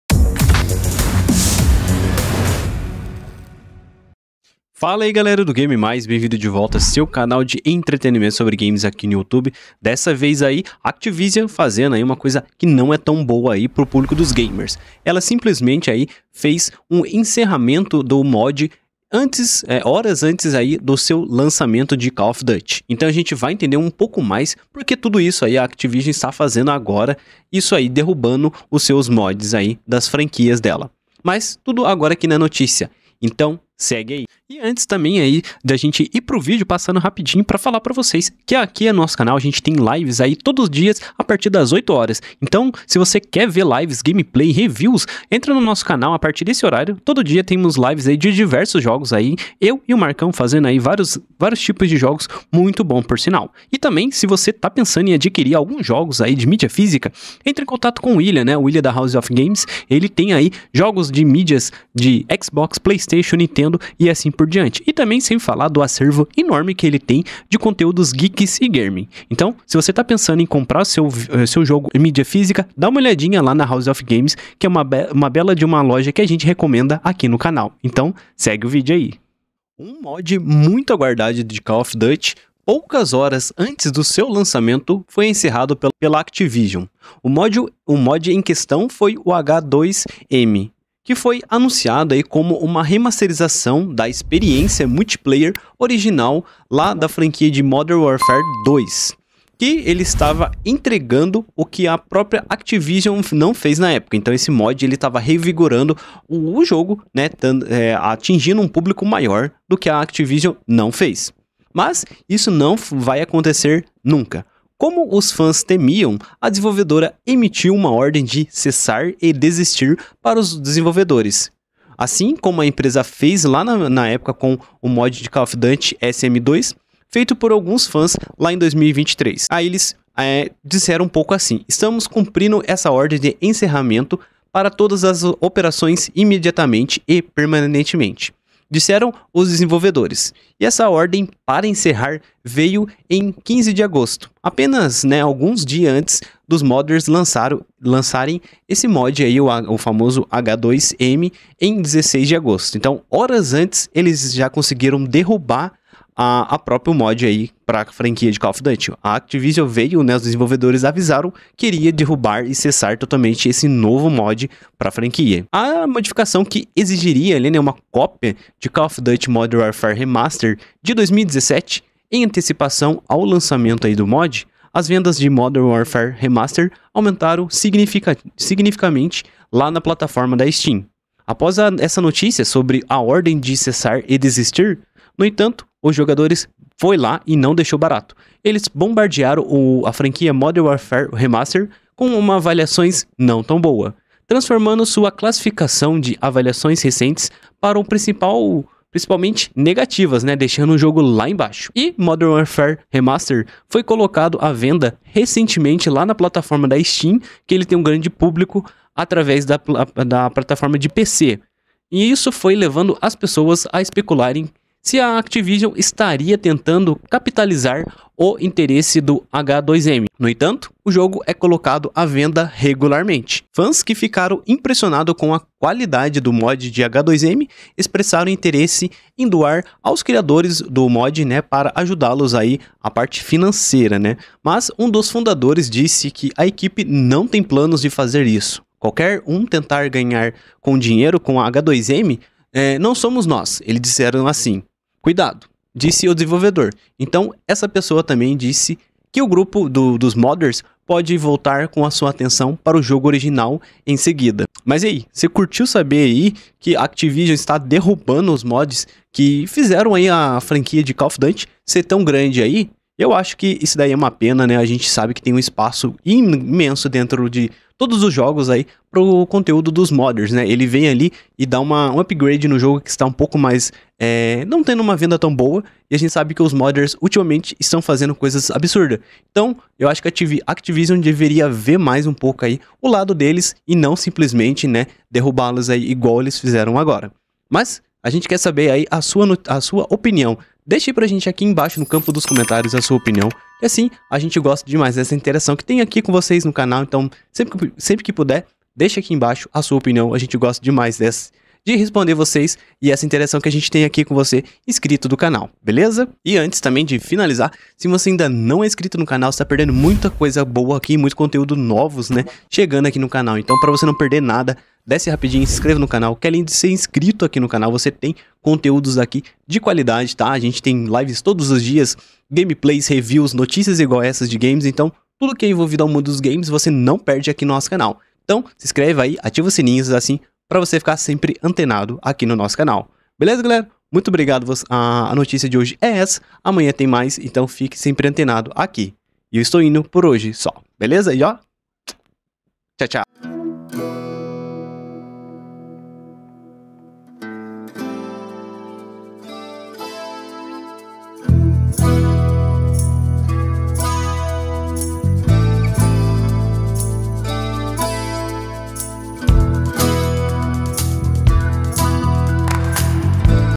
Activision ENCERRA o MOD para o Multiplayer de Modern Warfare 2 !
A Activision decidiu bloquear o popular mod H2M para Call of Duty: Modern Warfare 2 Remastered, que estava despertando, um enorme entusiasmo para os fãs do multiplayer frenético.

Planos Disponíveis
Prata: 4.90 por mês
Ouro: 19.90 por mês
Platina: 99.90 por mês